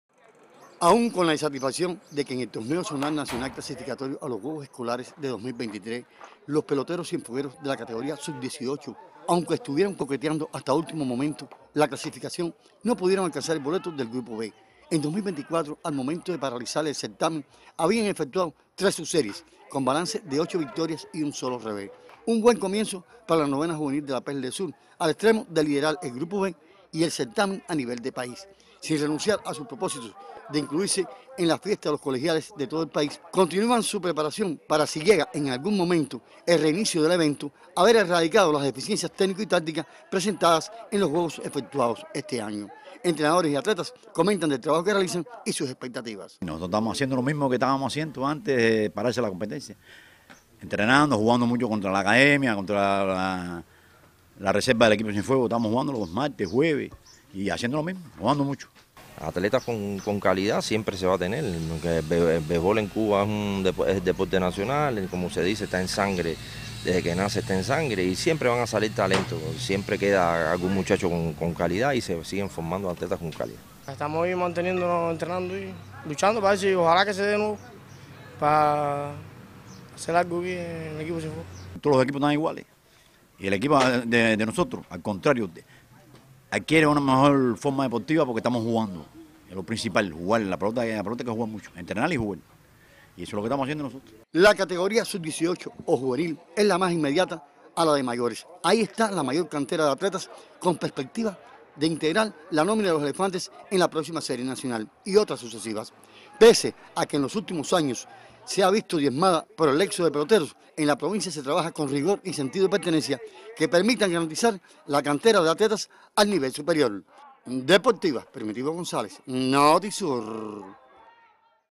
Continúa preparación equipo de béisbol juvenil de Cienfuegos
El equipo de béisbol juvenil de  Cienfuegos lideraba la zona y el país con ocho triunfos y un revés en las tres primeras subseries al momento de paralizar las competencias. Aun sin la certeza de cuándo se reanuden los encuentros mantienen su preparación.